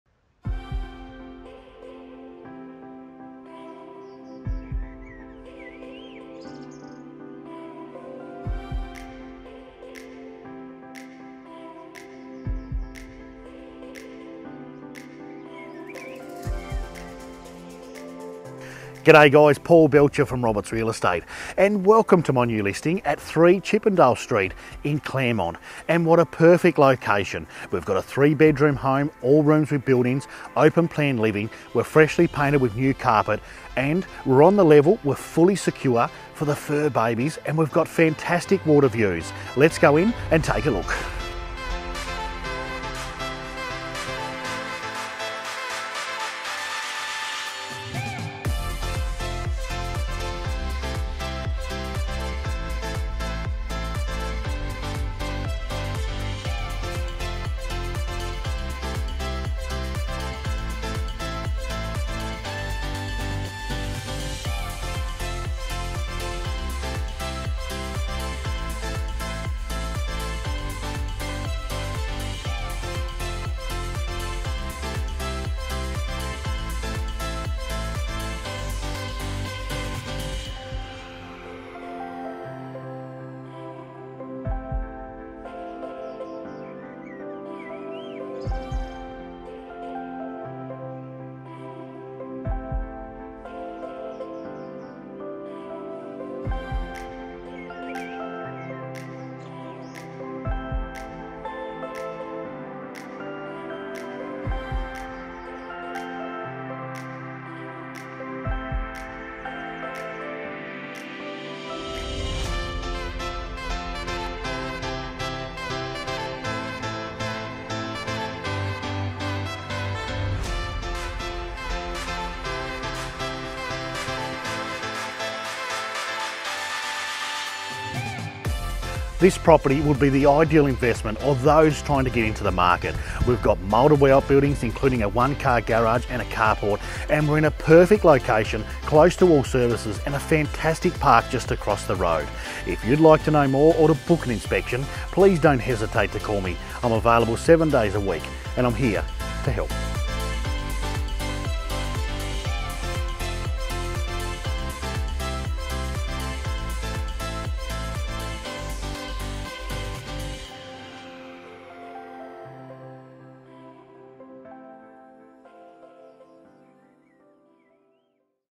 Roberts Real Estate Tasmania - 3 Chippendale Street Claremont
Take a look at this beautiful property located at 3 Chippendale Street in the heart of Claremont. This charming home boasts three cozy bedrooms and is clad throughout. The house has been freshly painted and new carpet has been installed, giving it a modern look and feel. The home is surrounded by established gardens and the yard is fully secure, making it perfect for families with kids and pets. There is a garage and carport, along with a garden shed and bird aviary, providing ample storage space for all your needs. One of the best features of this property is the fantastic deck that offers amazing water views. You can relax and enjoy the tranquil atmosphere while taking in the beautiful scenery. The property is also conveniently located close to all the services you need, making it a perfect place to call home.